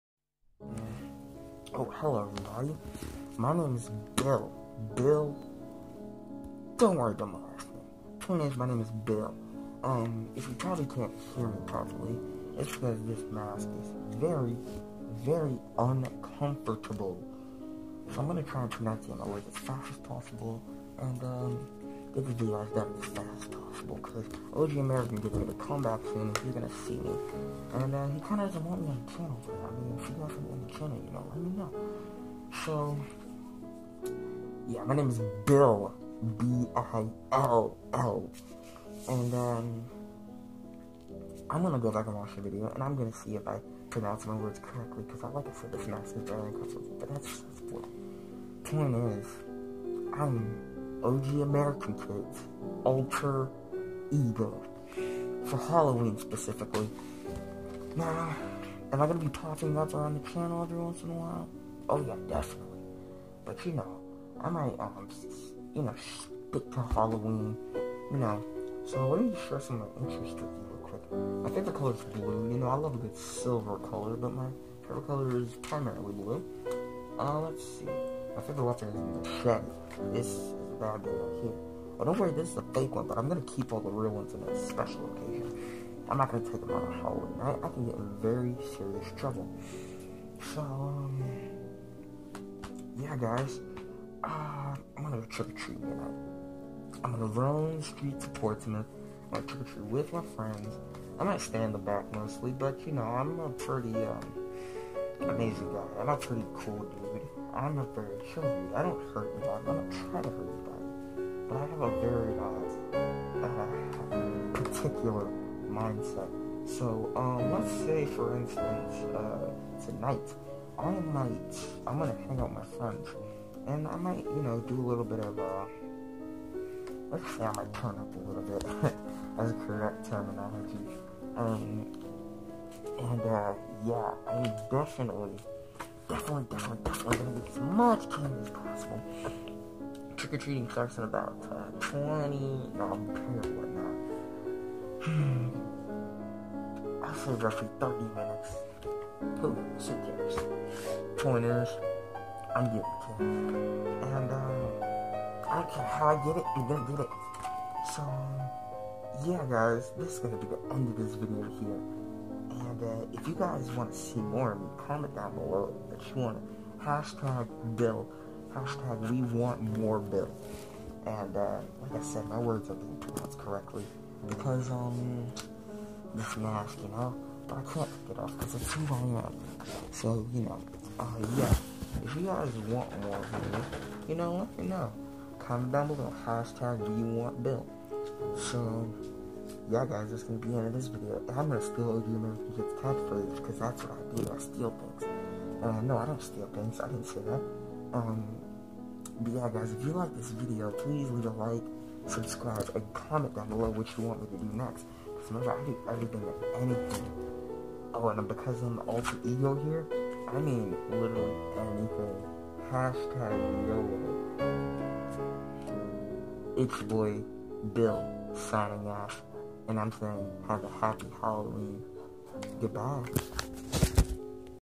The Birth Of Bill!! Halloween skit special
Guys I decided to let my alter ego take over for Halloween cause it's his favorite event of the year. And this is also a way to introduce someone new to the channel. So if you like the idea of a fresh face let me know and you will see more of Bill in the future. Stay safe and have a happy Halloween 🎃!!